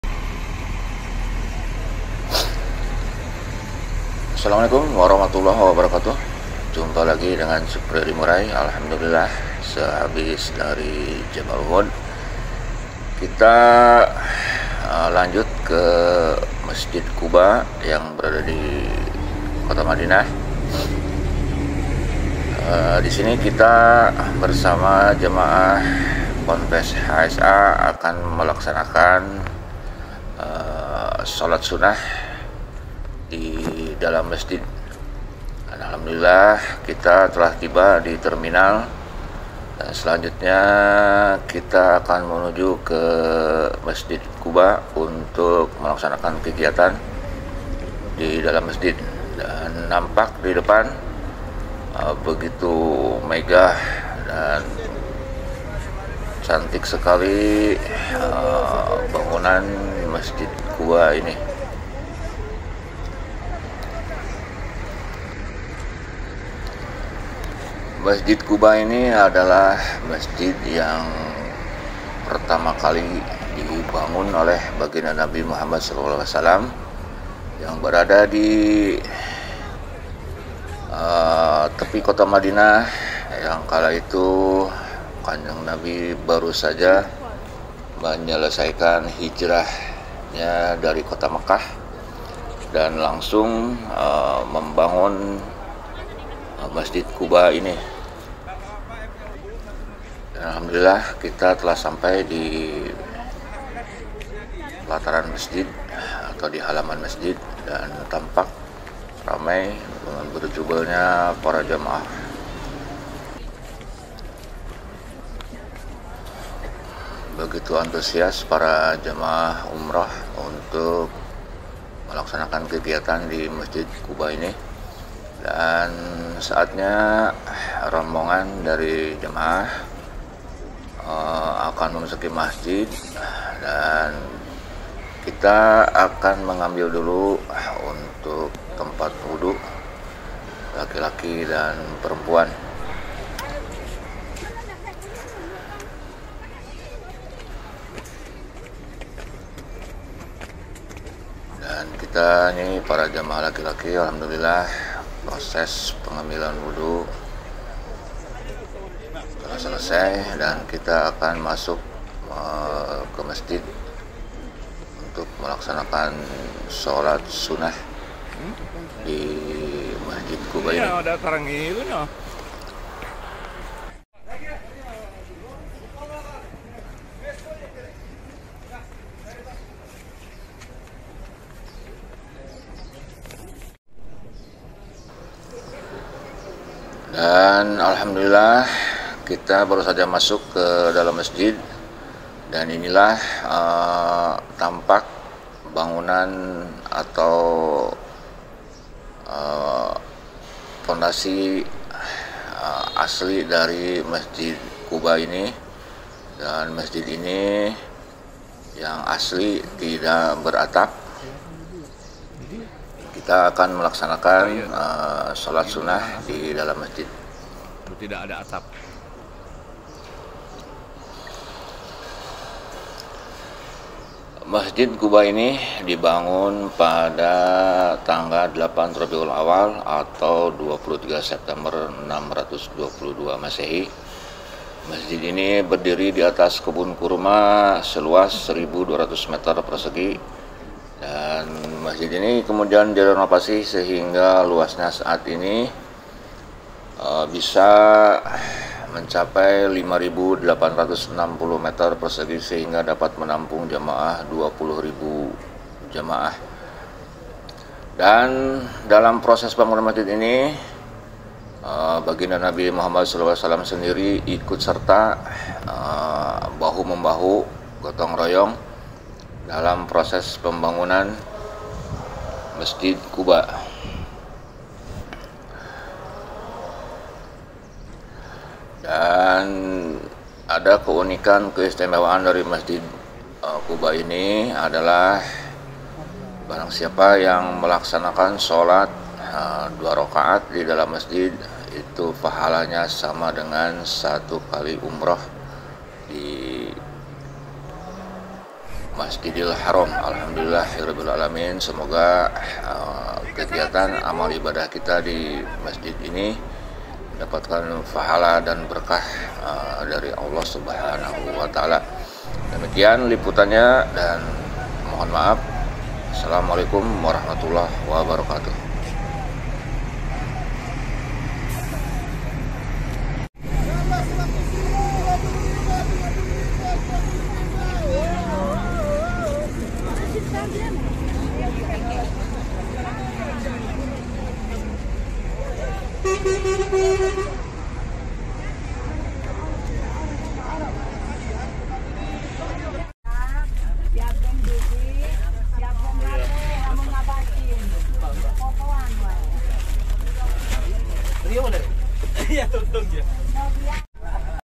Inilah Mesjid Quba..mesjid pertama yang di bangun Rasul di madinah
vidio city tour jamaah umroh ponpes hsa ke mesjid Quba madinah